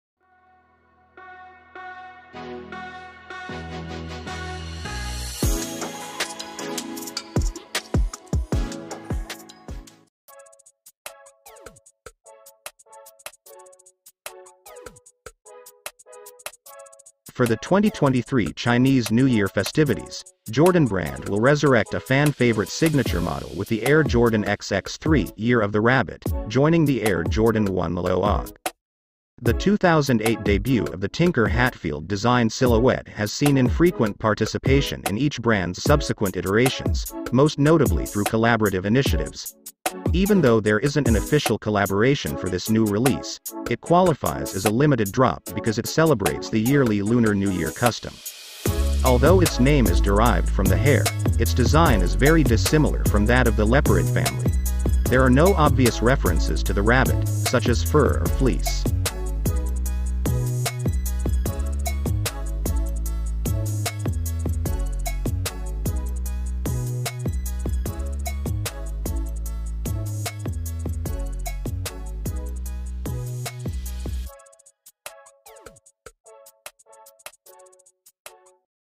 YEAR OF THE RABBIT 2023 Air Jordan XX3 the Return $260 Cop or Drop?
Air Jordan XX3 “Year of the Rabbit”

Style Code: FB8947-001
Color: Light Silver/Challenge Red-Ivory-Metallic Gold-White
Retail: $260

For Men/Women
Upcoming
Detailed Look